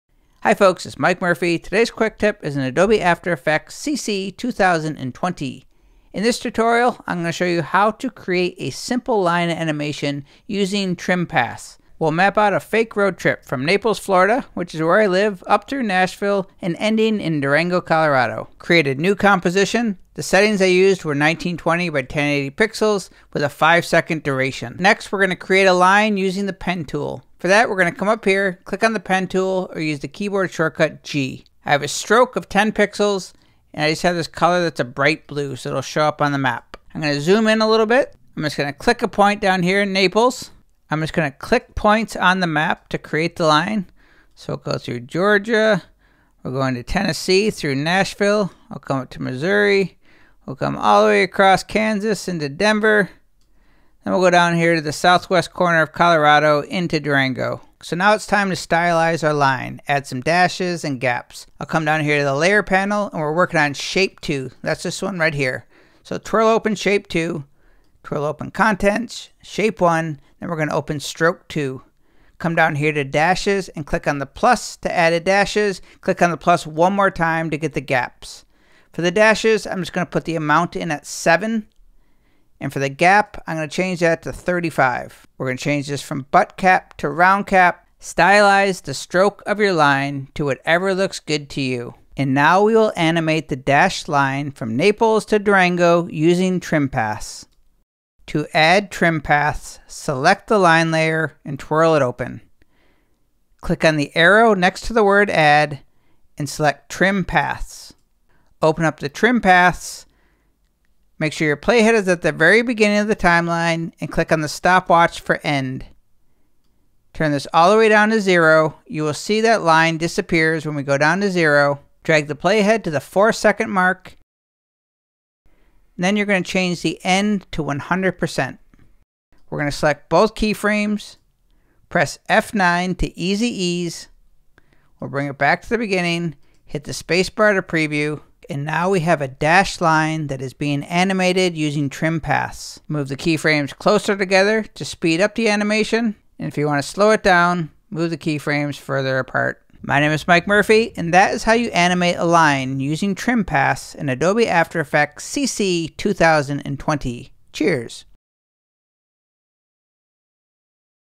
After Effects: How To Use Trim Paths to Animate a Dashed Line on a Map
After Effects CC 2020: Animate a Dashed Line on a Map using Trim Path

How to Make a Stroked Line in Adobe After Effects:
1. Create a New Composition (cmd + n)
2. Select the Pen Tool (G)
3. Turn off Fill
4. Turn on Stroke (choose color and weight)
5. Click to make points and when you stop you will have a line

How To Add Dashes Adobe After Effects CC 2020:
1. Go to Layer Panel
2. Twirl down arrow next to Shape Layer
3. Twirl down Contents
4. Twirl down Stroke 1
5. Click + to Add Dashes
6. Adjust Stroke Properties to Taste

How To Animate Using Trim Paths:
1. Go to Layer Panel
2. Twirl down arrow next to Shape Layer
3. Click Arrow Next to Add
4. Click on Trim Paths
5. Move Playhead to beginning of timeline
6. Click Stop-Watch for End
7. Change Percentage to 0
8. Drag Playhead to 4 Seconds
9. Change Percentage of End to 100%
10. Select Both Keyframes (Drag over them)
11. Press F9 to add Easy Ease to smooth out the animation
12. Move keyframes closer together to speed up animation
13. Move keyframes further apart to slow down animation